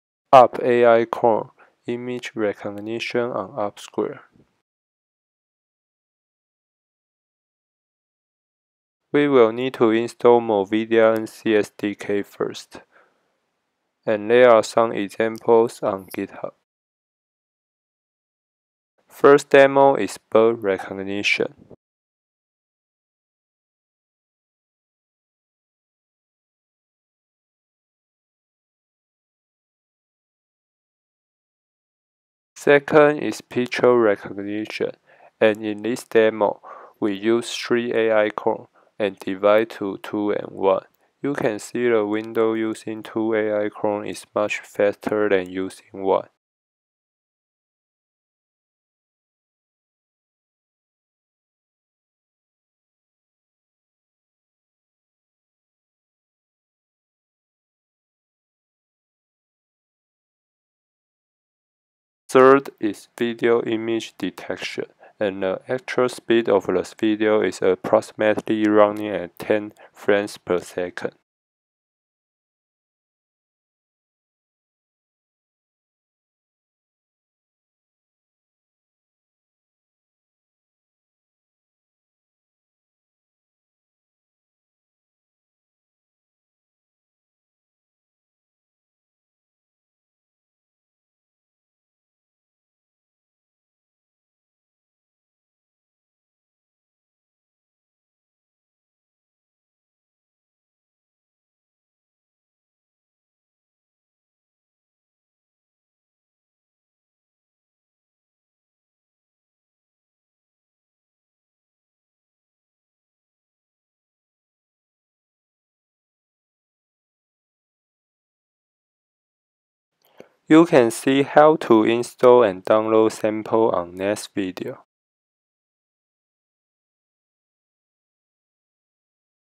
UP AI Core- image recognition on UP squared
UP AI CORE-The first embedded ultra-compact Artificial Intelligence processing card for on the edge computing.
In this video, we will show some demo running AI core on UP squared(Ubuntu 16.04)